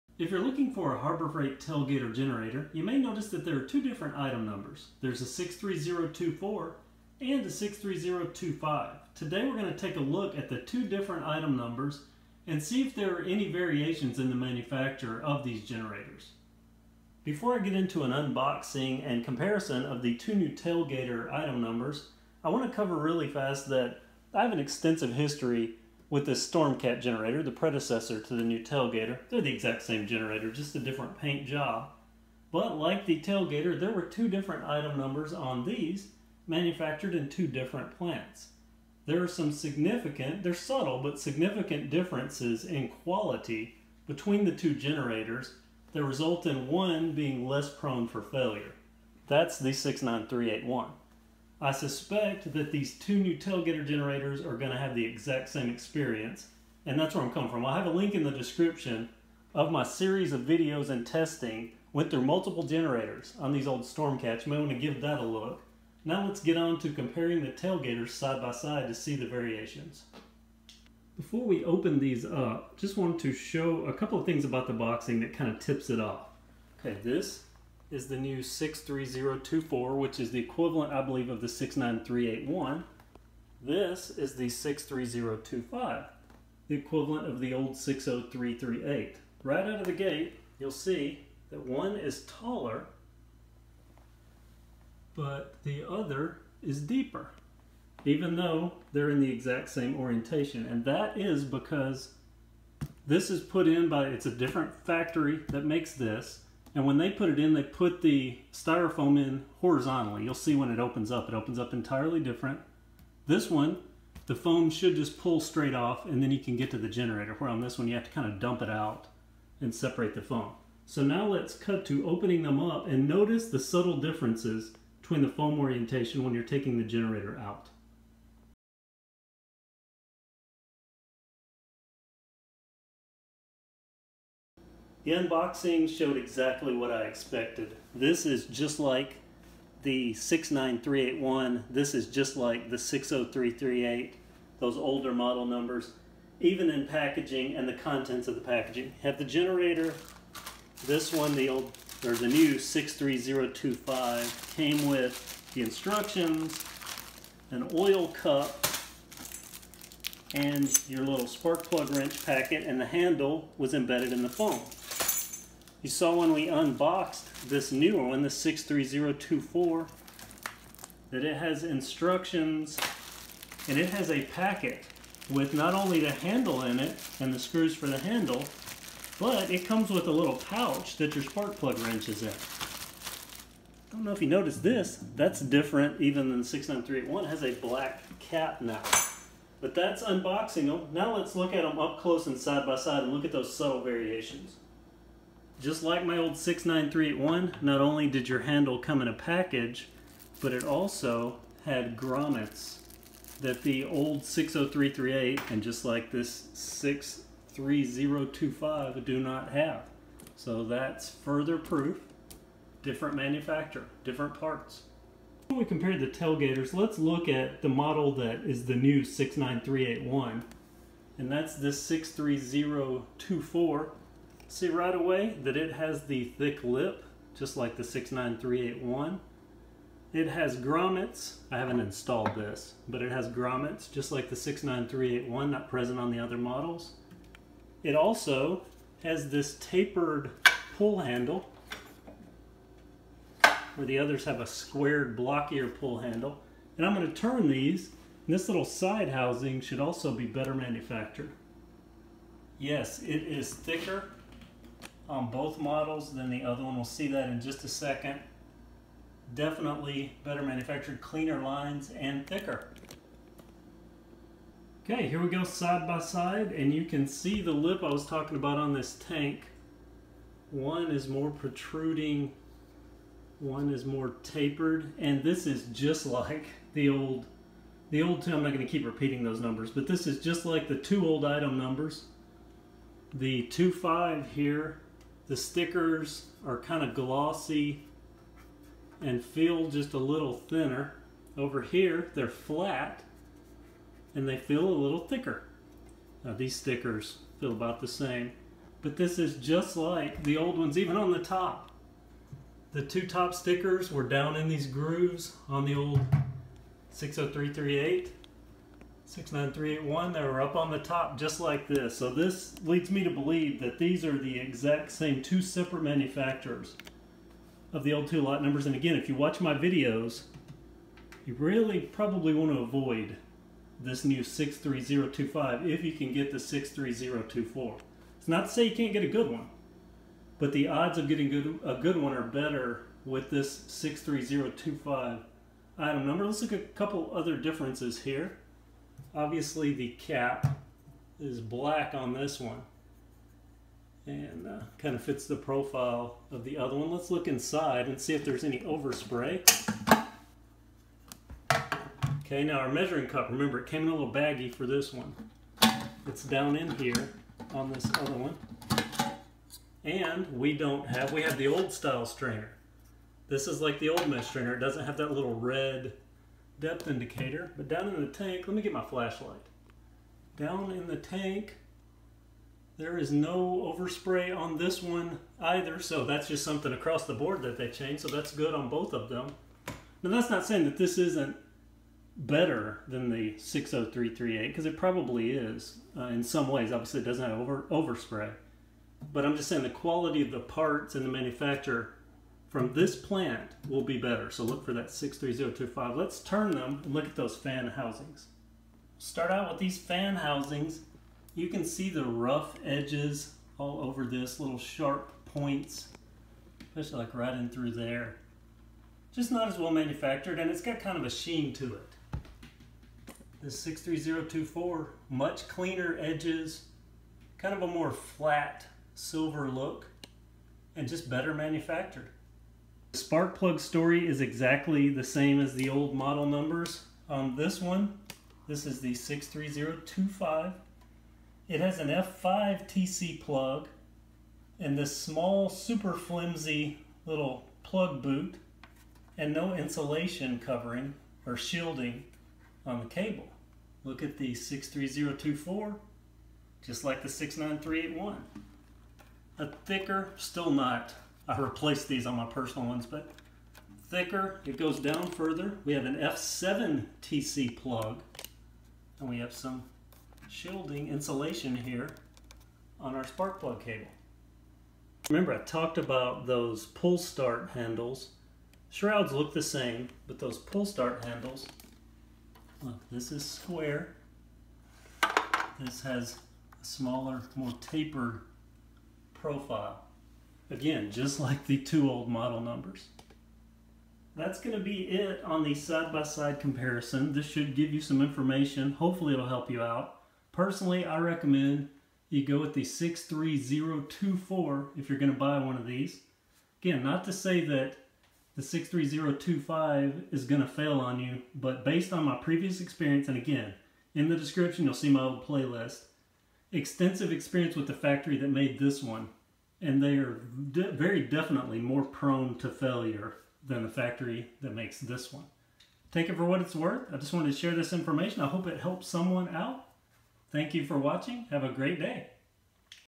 TailGator generator 63024 vs 63025 comparison - Which Harbor Freight Generator to buy?
Harbor Freight has two item numbers for their TailGator 900 watt portable 2-cycle generator. There is a 63024 and a 63025. These different item numbers are manufactured at different facilities and have a few quality control and part differences as well as packaging differences.

I have previously covered the Harbor Freight StormCat 900 watt portable generator extensively. There is a playlist available that covers the testing and results of my experience with the StormCat:

Based on my experience, it appears that the new TailGator #63024 is the equivalent of the older StormCat 69381, and that this would be the one to select when choosing a generator. That's not to say that you would have issues with the 63025, as it appears to have improved over the older 60338 quality, but still has lower quality finishing than the 63024.

In short,

StormCat 60338 = TailGator 63025
StormCat 69381 = TailGator 63024 (recommended)